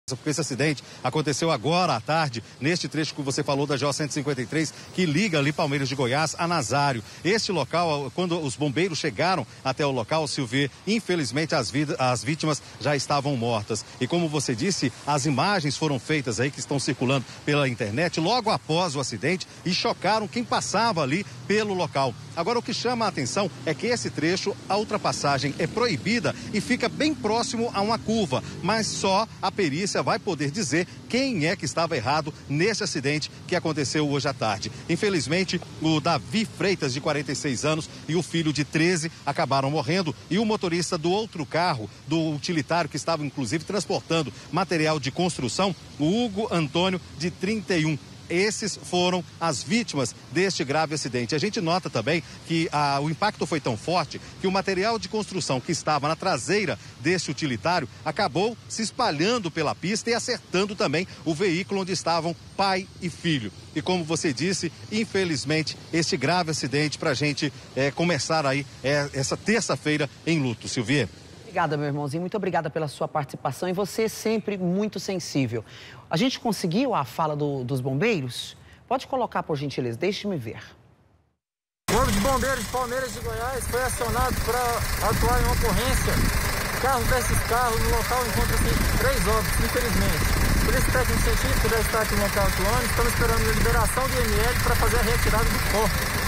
TRAGÉDIA: 3 PESSOAS MORREM EM BATIDA VIOLENTA NA GO-156
TRÊS PESSOAS MORRERAM NUM GRAVE ACIDENTE NA GO 156, ENTRE PALMEIRAS DE GOIÁS E NAZÁRIO. PAI E FILHO QUE ESTAVA NUM CARRO E O MOTORISTA DO OUTRO VEÍCULO.